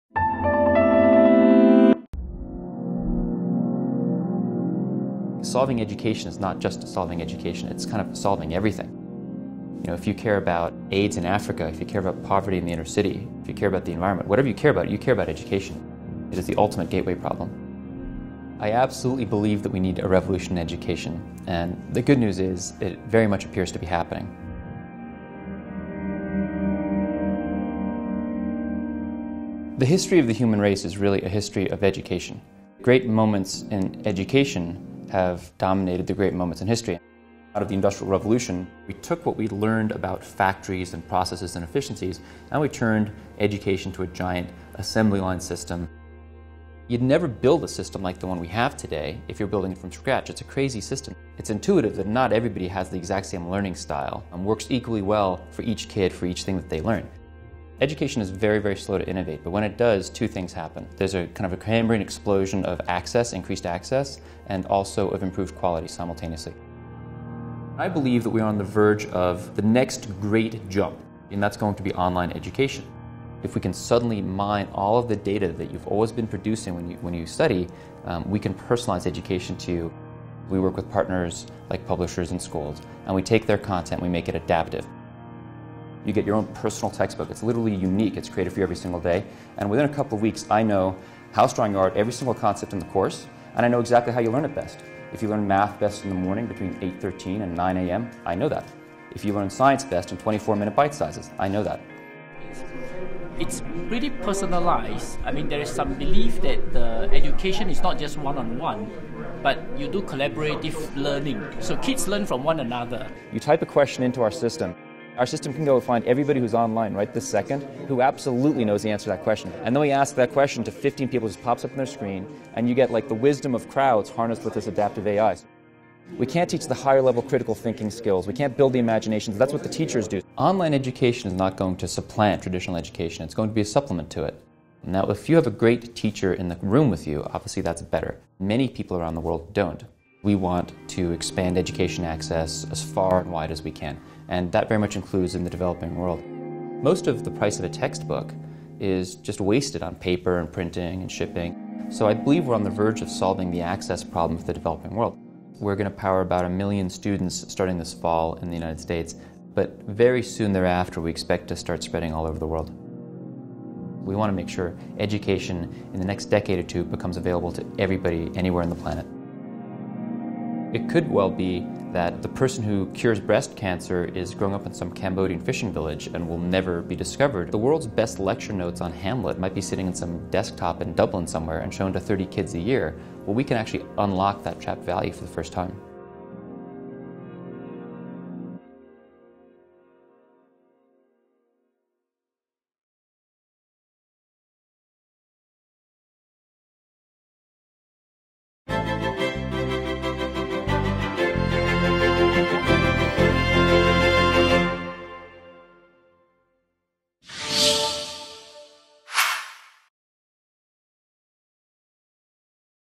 Expanding & Personalizing Online Education-A World Economic Forum Discussion-Ideas @Davos-WIRED Live
Is the education system ready for a revolution? In this World Economic Forum discussion Jose Ferreira, CEO and founder of Knewton, explains how a more personalized, adaptive program would increase access to education and improve the quality of teaching.

Starring: Jose Ferreira